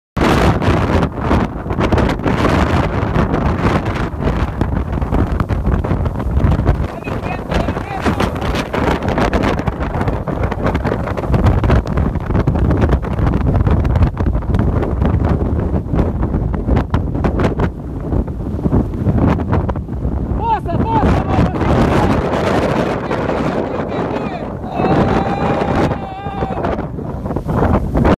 Ya esta llegando.. Huracan Irma en Miami, Florida
Ya se esta asomando el Huracán Irma en Miami, Florida
Aquí un breve vídeo del cual muestra una escalofriante escena de lo que sera el huracán.